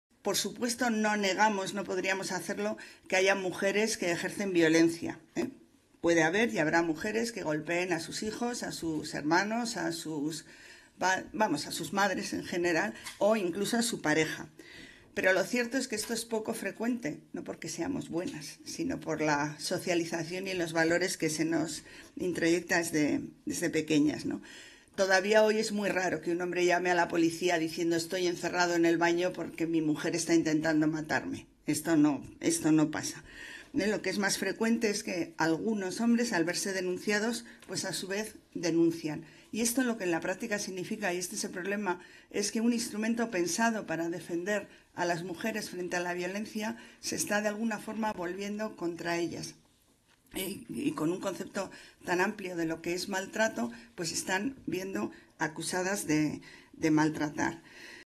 Contradenuncias: Instrumento pensado para defender a las mujeres se está volviendo en contra
Extracto de la presentación del estudio becado por Emakunde que ha analizado la experiencia de las mujeres en los procesos de detención policial. Ponente: Miren Ortubay Fuentes.

Ponencia de la directora de Emakunde, Izaskun Landaida Larizgoitia (2 min 25 seg)